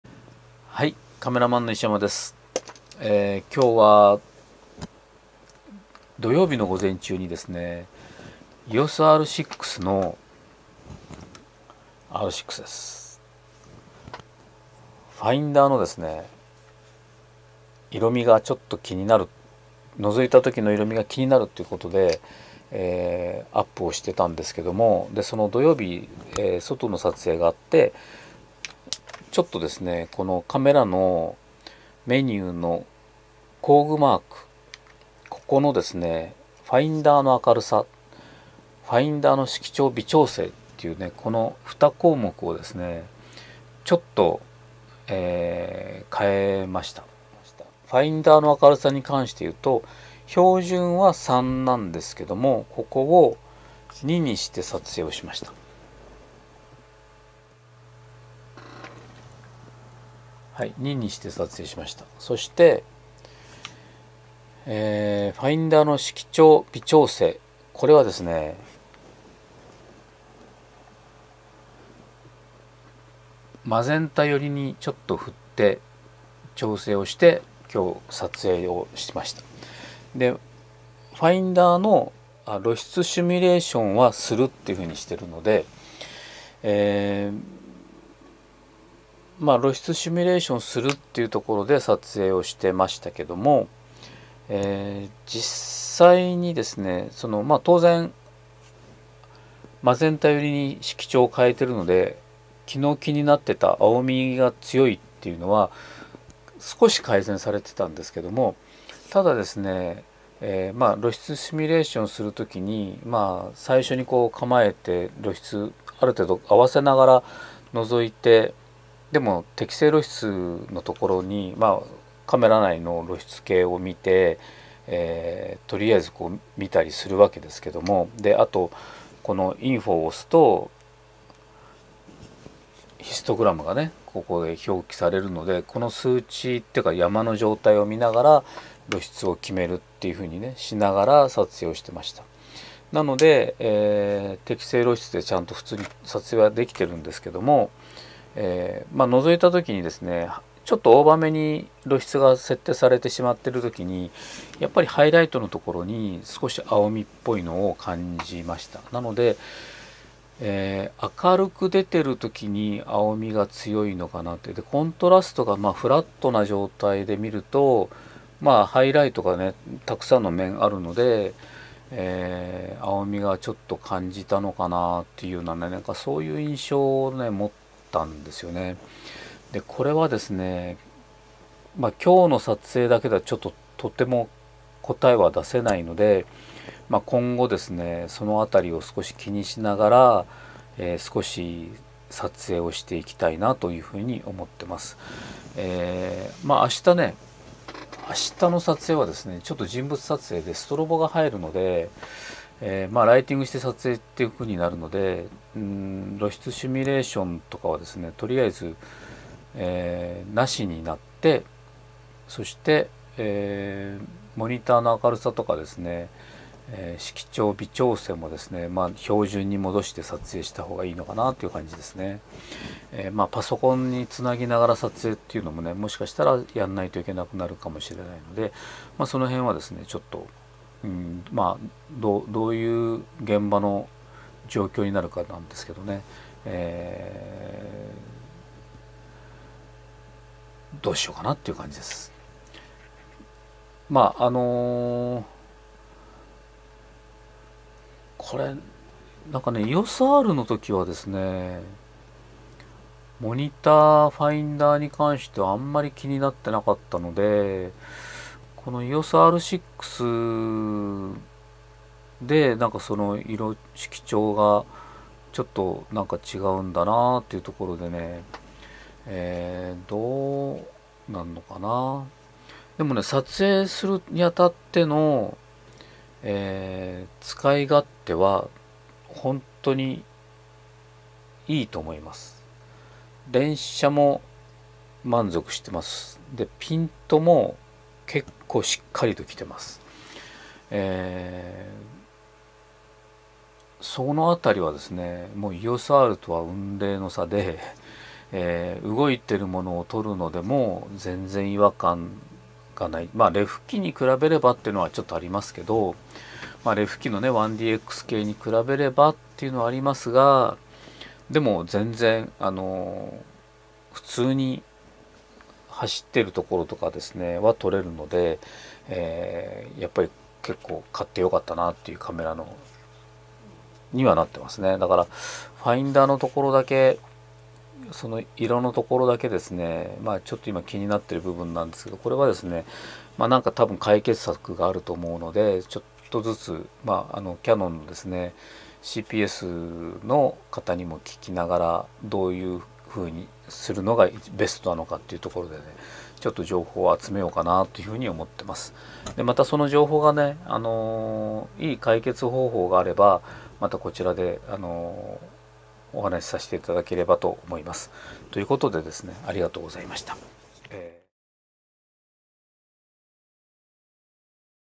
Canon EOS R6ファインダー色調微調整の件
Canon EOS R6のファインダーの青みが強く感じるので、ファインダー色調微調整とファインダーの明るさ調整をやってみました。